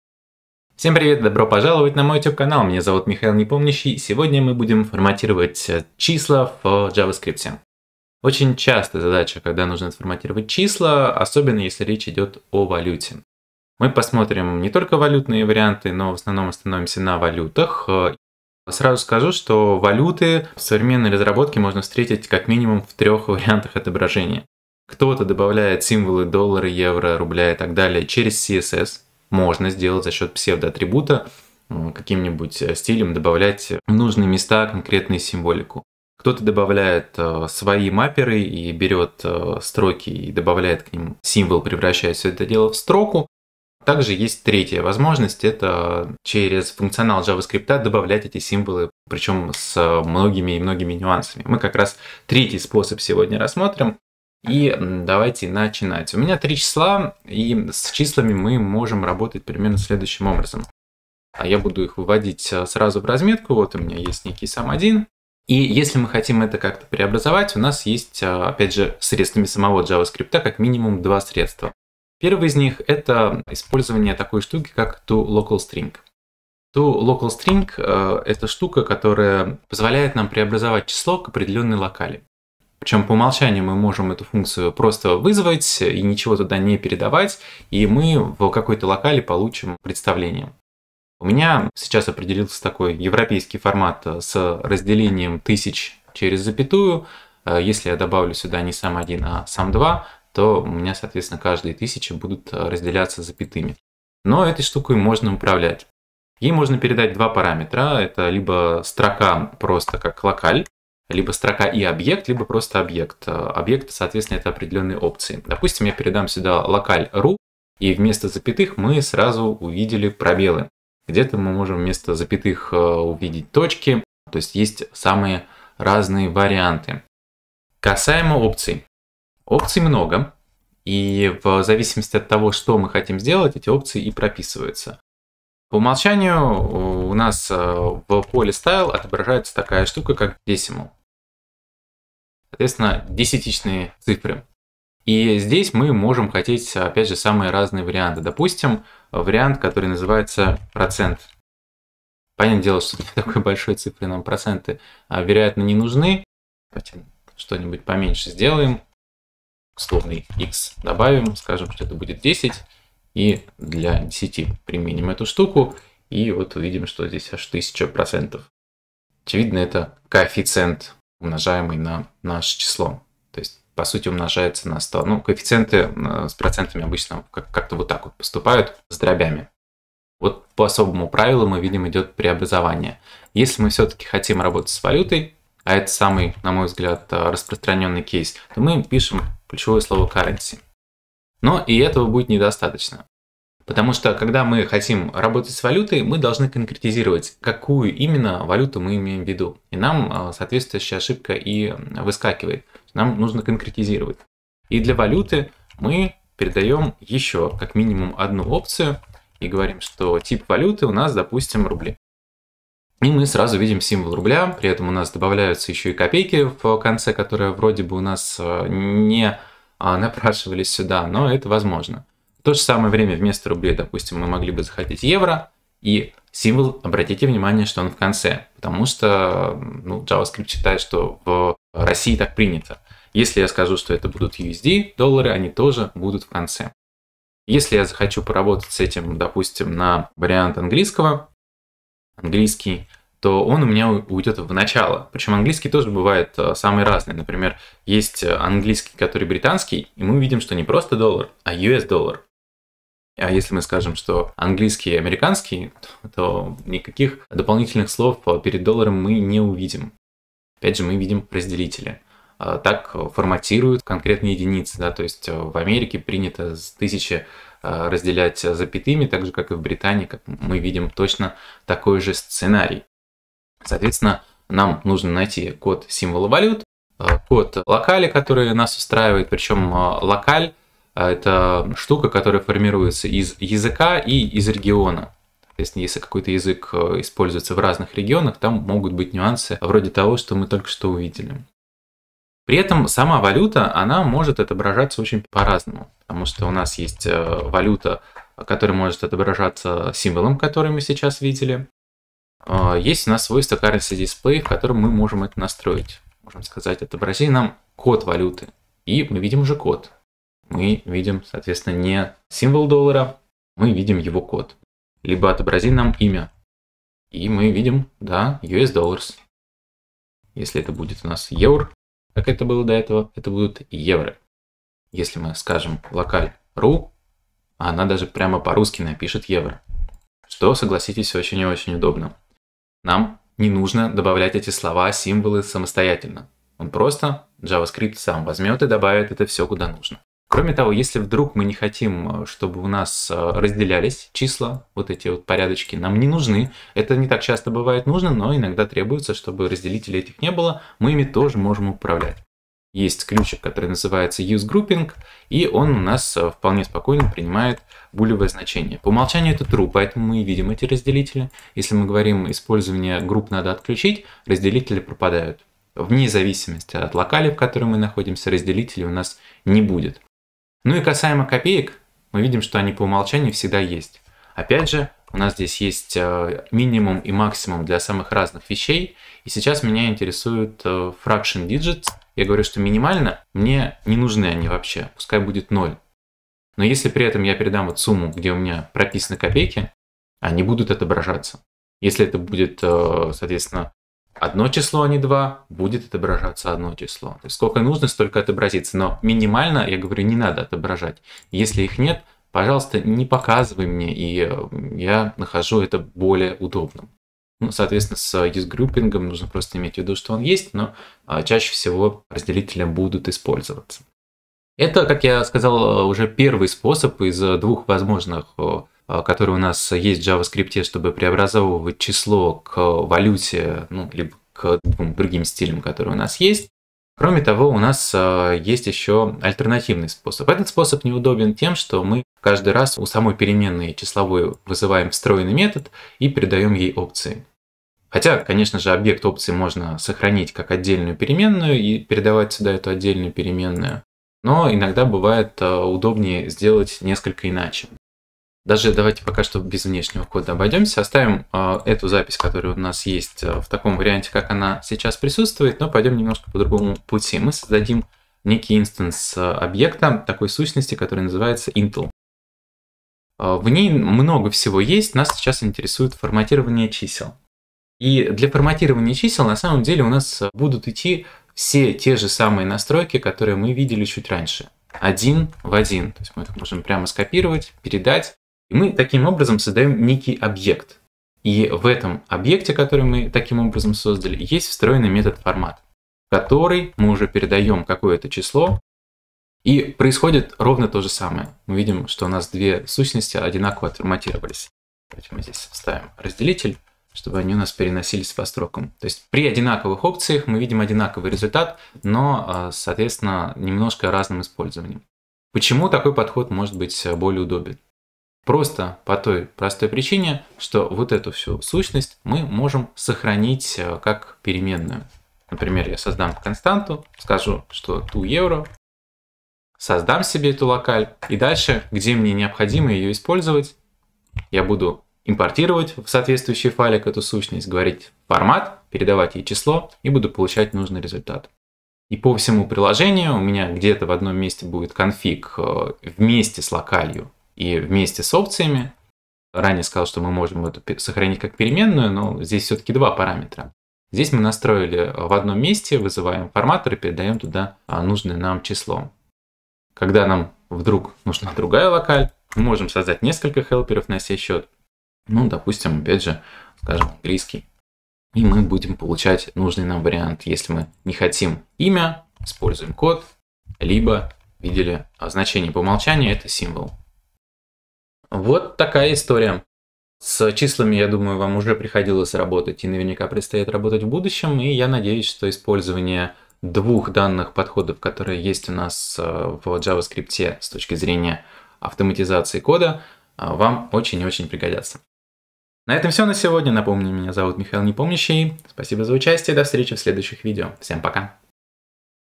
Форматирование чисел в JavaScript
Декларативный способ форматирования числе в JavaScript. Разбираем встроенный в числа метод toLocaleString и конструктор Intl.NumberFormat.